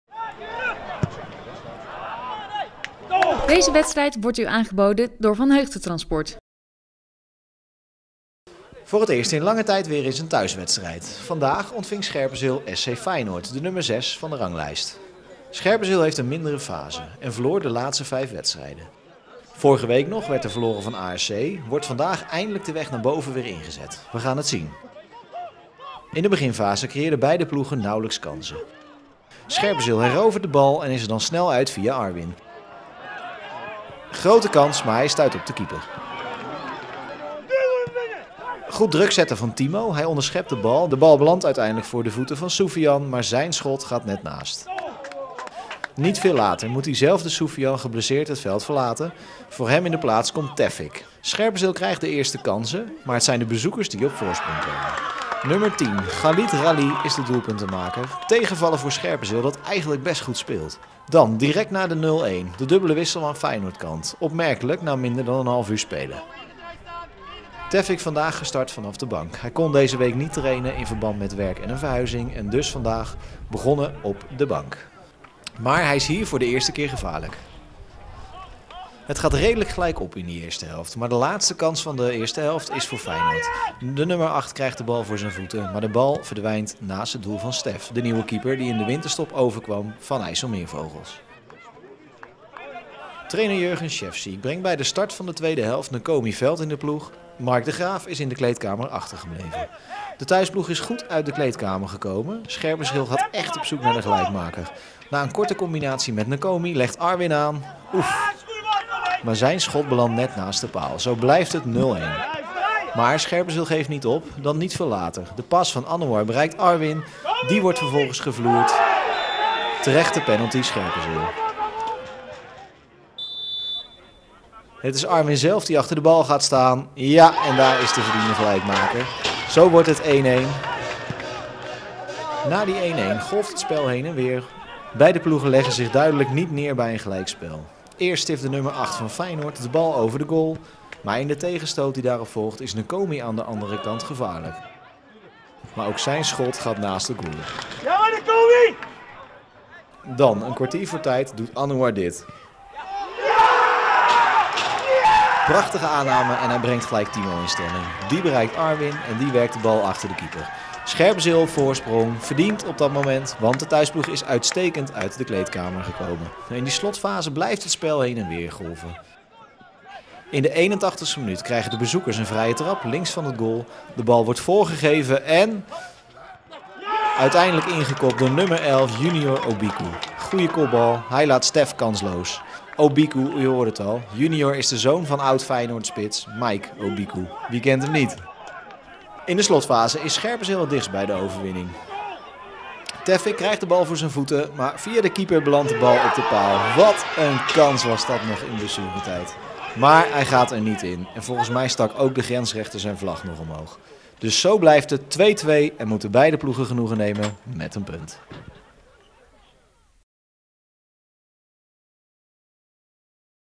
Samenvatting | v.v. Scherpenzeel - SC Feyenoord | Hoofdklasse A Zaterdag | 12-2-2022
Zaterdag 12 februari 2022 speelde op Sportpark De Bree-Oost v.v. Scherpenzeel tegen SC Feyenoord in de Hoofdklasse A Zaterdag. Alle goals zijn terug te zien in deze samenvatting.